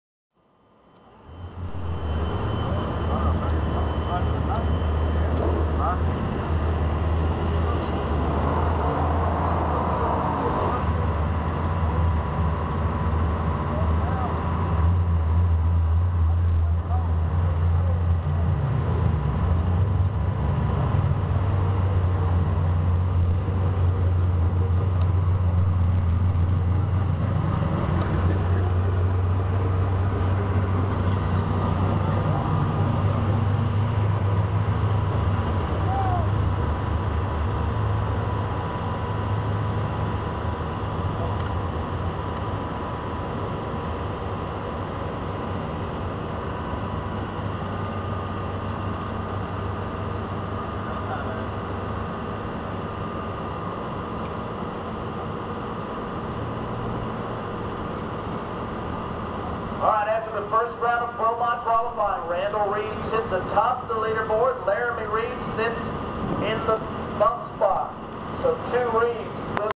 dragggs kennadale tx 029.AVI
Gaylon smith's 57 promod getting it ready for the next race.